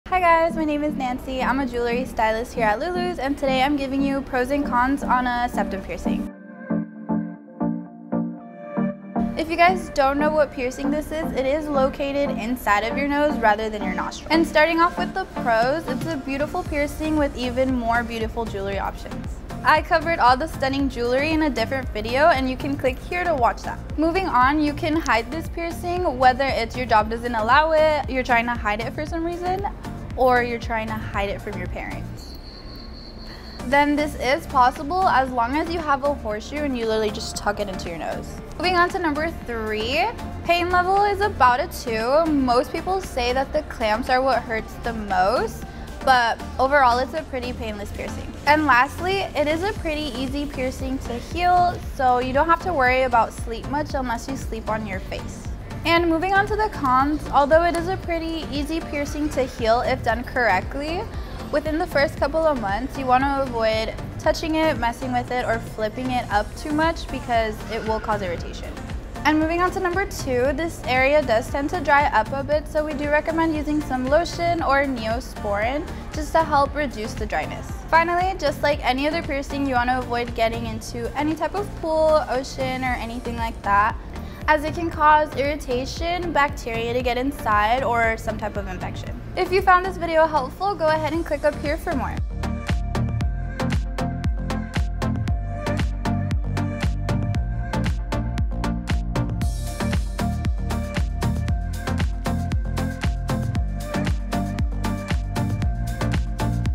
Pros & Cons of Getting A Septum Piercing 😳
Thinking of getting a septum piercing well Nancy gives you some personal Pros and Cons!

Featured Jewelry:
✨👂14K Crystal & Opal Hinged Ring

Our Jewelry:
✨👂14KT Gold Collection